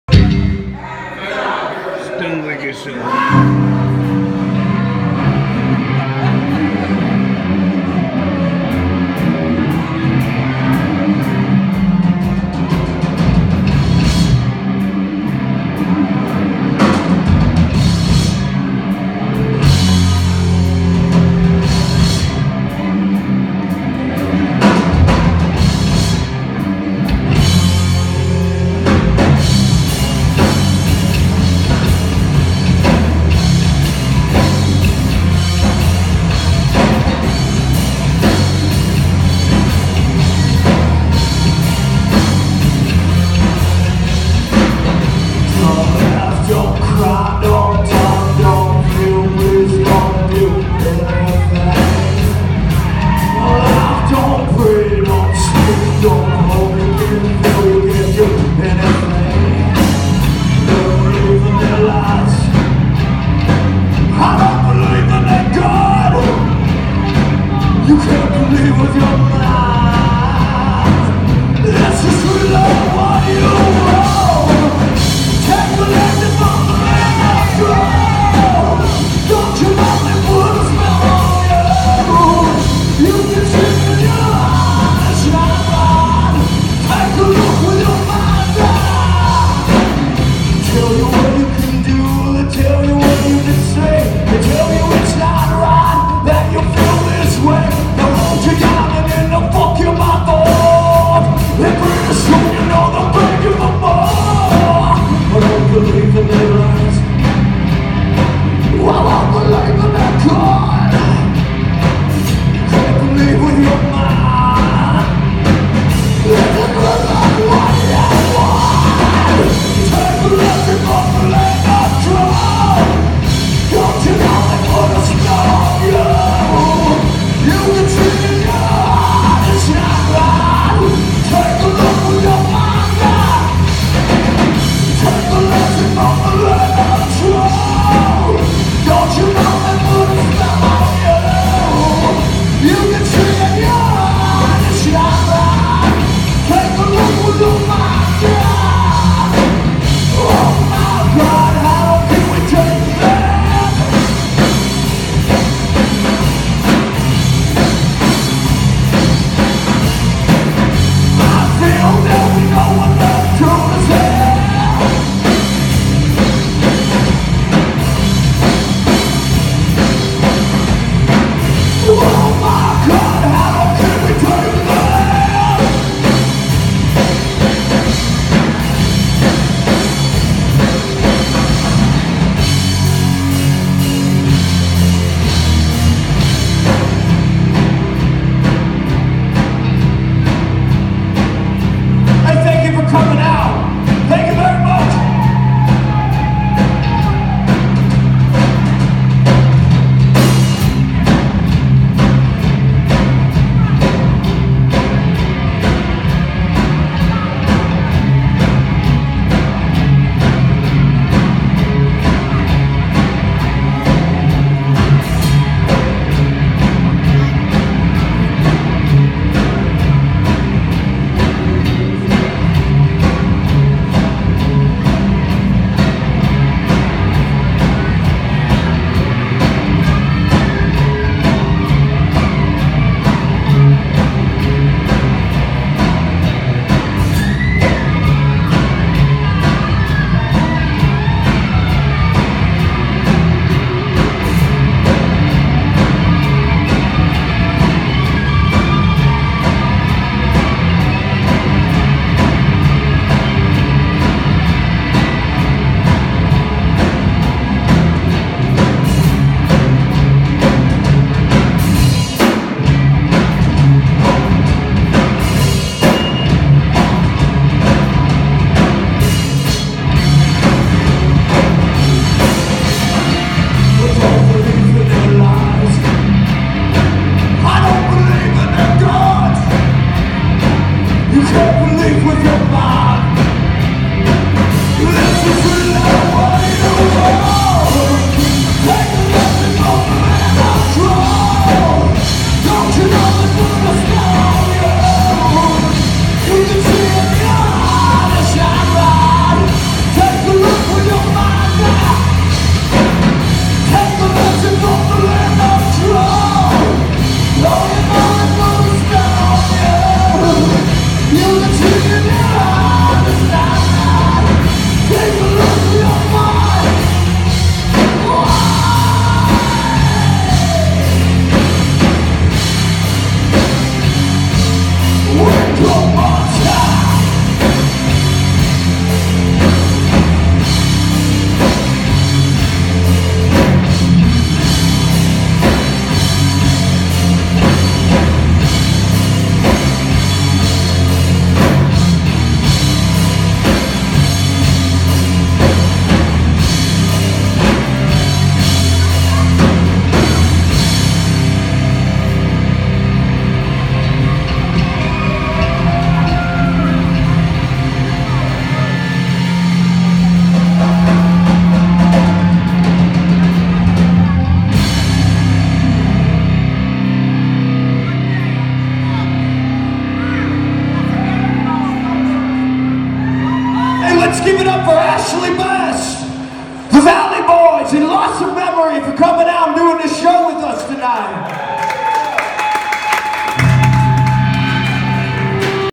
Stone Wicked Souls
Oct 2, 2015 Jewett Sportsman's Club.Sound and recording provided by War Dog Sound.These Guy's Rock \,,/